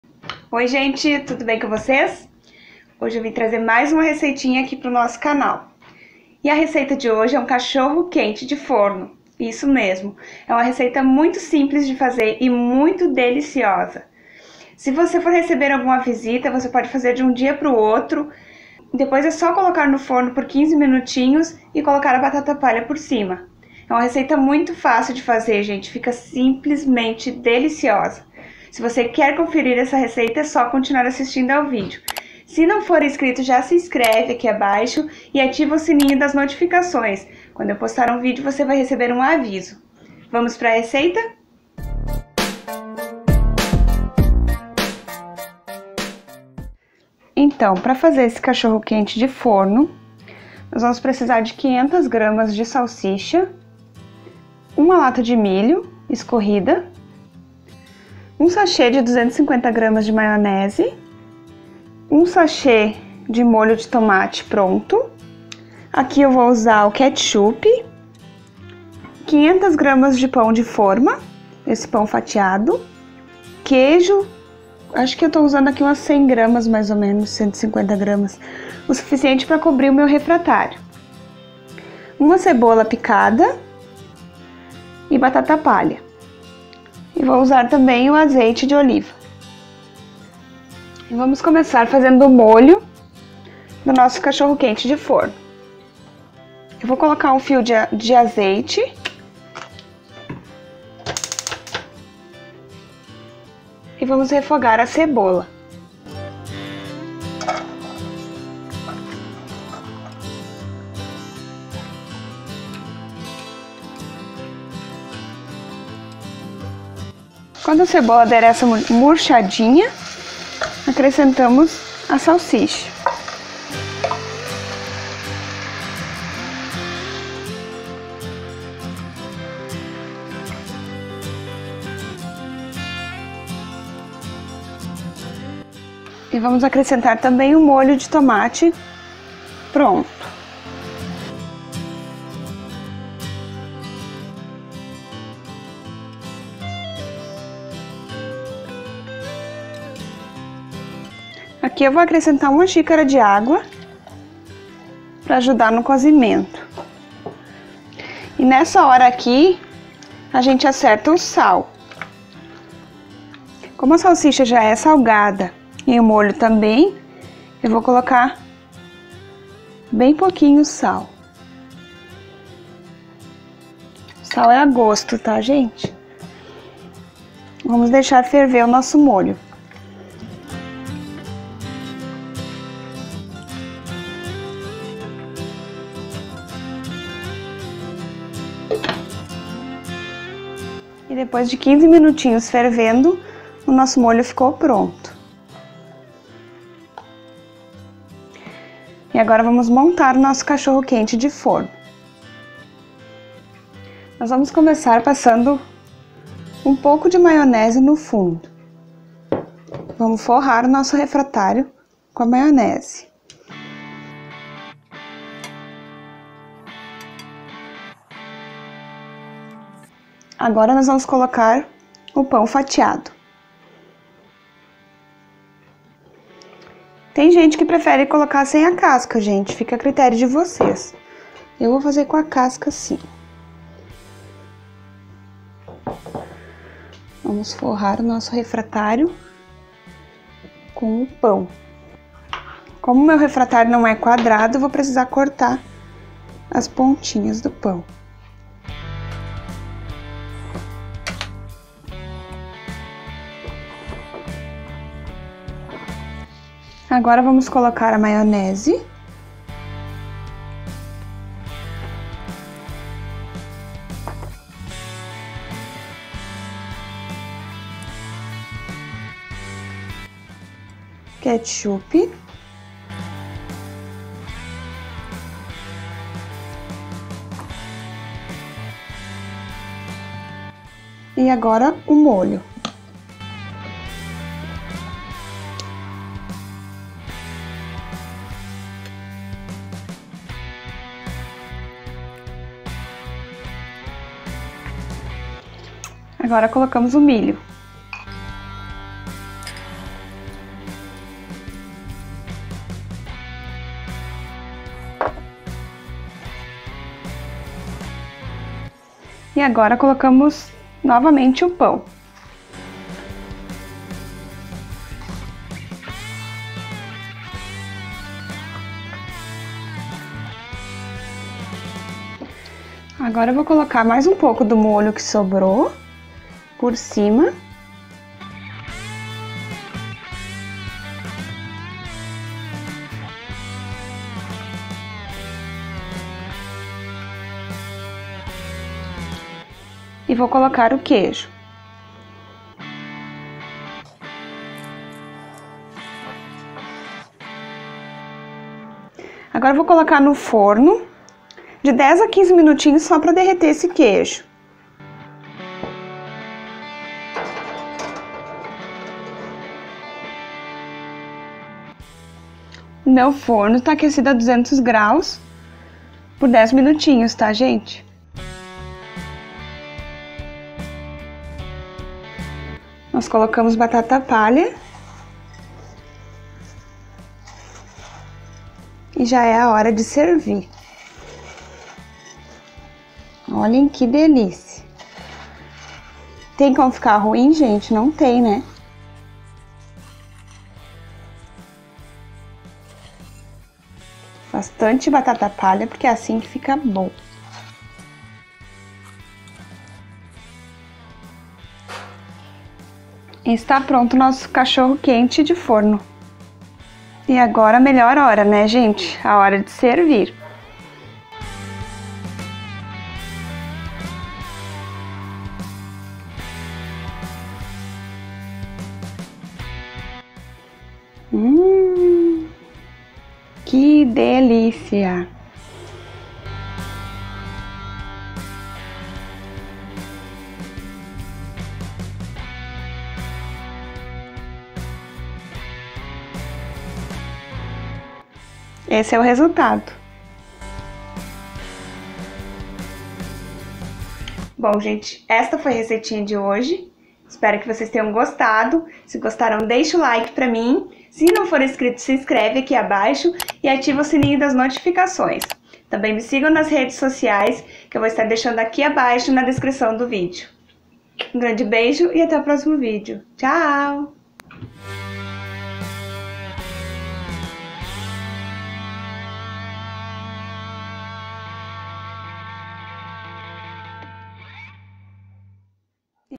CACHORRO QUENTE DE FORNO MAIS FÁCIL DO MUNDO | RECEITA SIMPLES E DELICIOSA!
Oi gente!! Hoje eu trouxe a receita do CACHORRO QUENTE DE FORNO MAIS FÁCIL DO MUNDO!!
Uma receita muito simples, fácil de fazer e muito deliciosa!

INGREDIENTES

500 g de salsicha
1 sachê 200g de maionese
1 lata de milho
1 sachê de molho de tomate
150g de queijo fatiado
500g de pão fatiado
180g de batata palha
1 cebola média picada
Ketchup